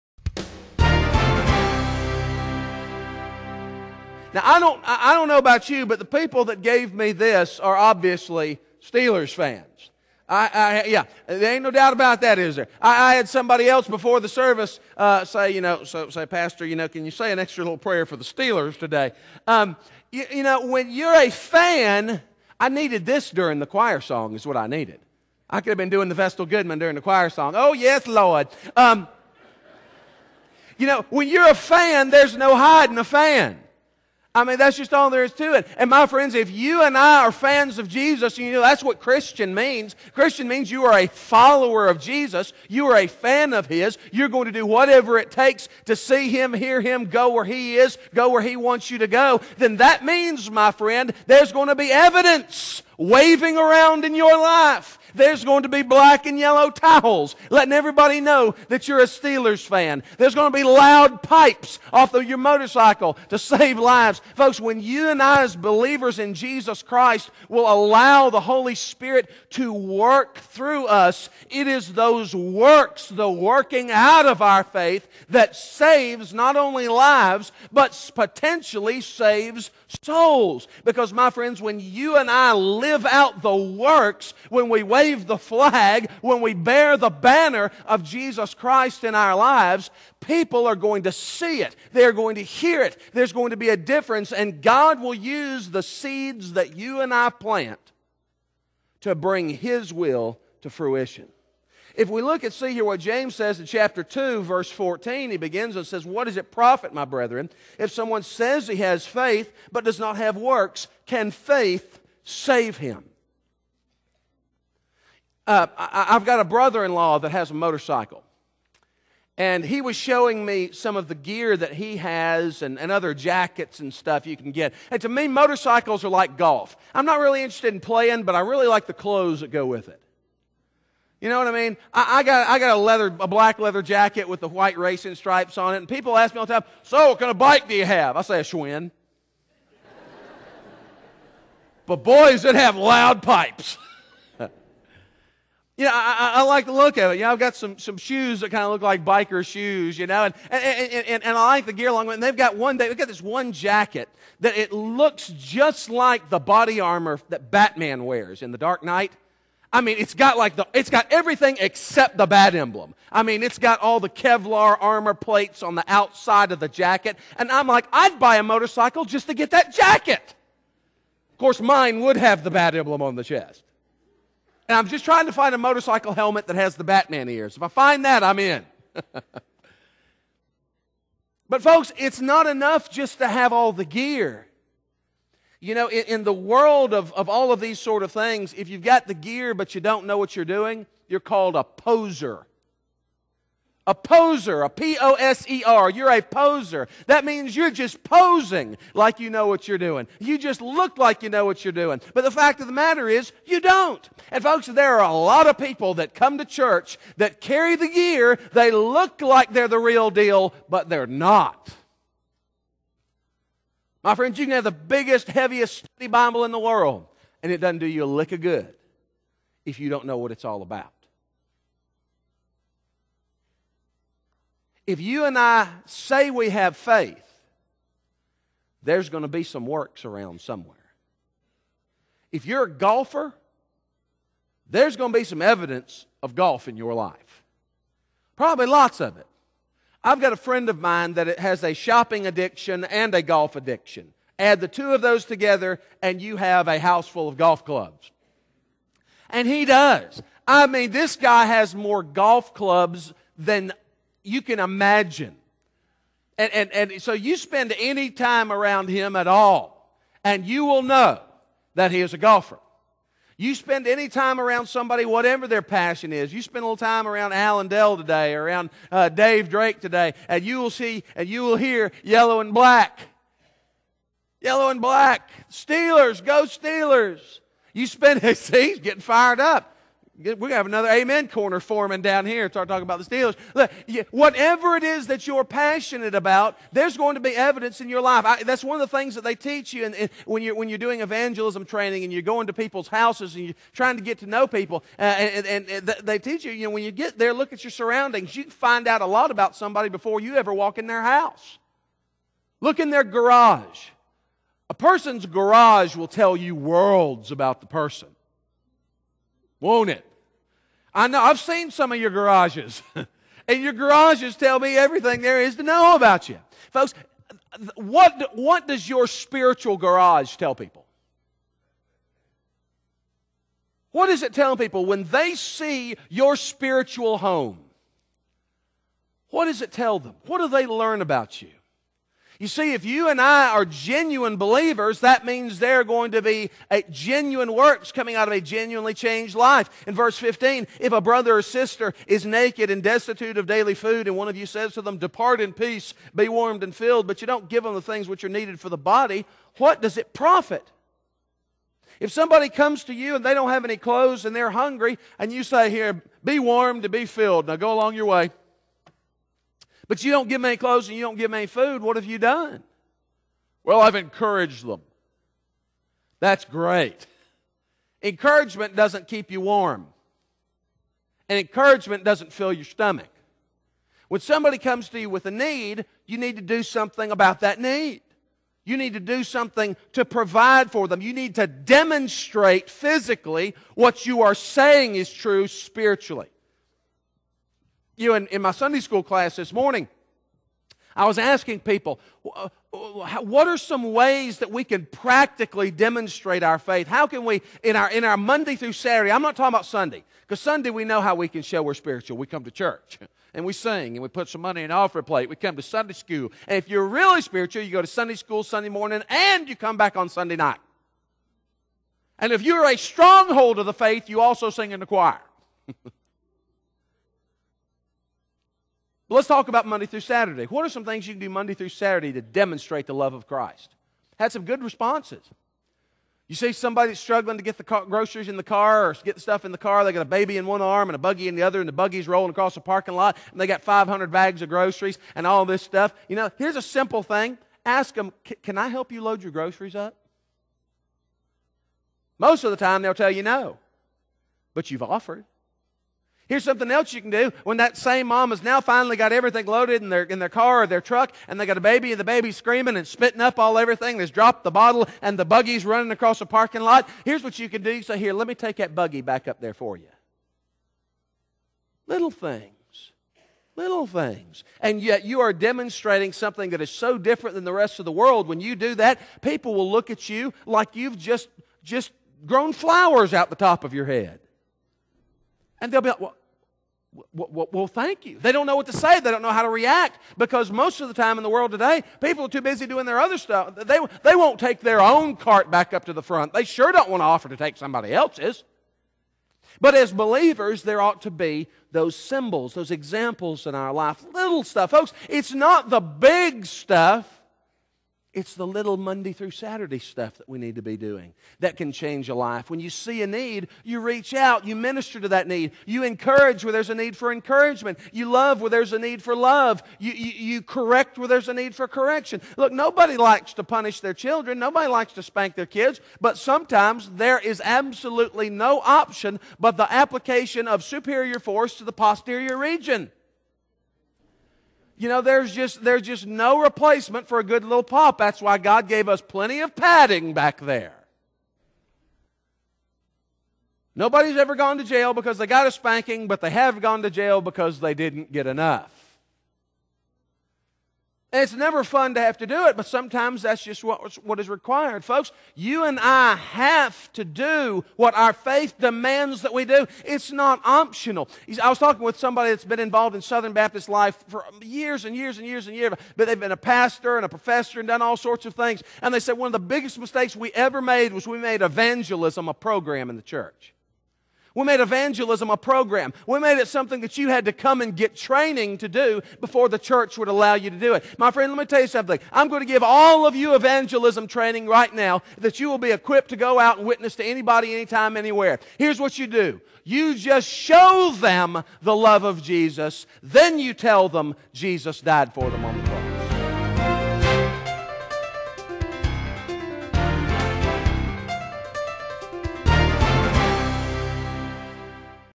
"Is Your Motor Running?" sermon by Pastor Marcus Buckley, Riverside Baptist Church in Greer, SC
This is an excerpt from a series in the book of James.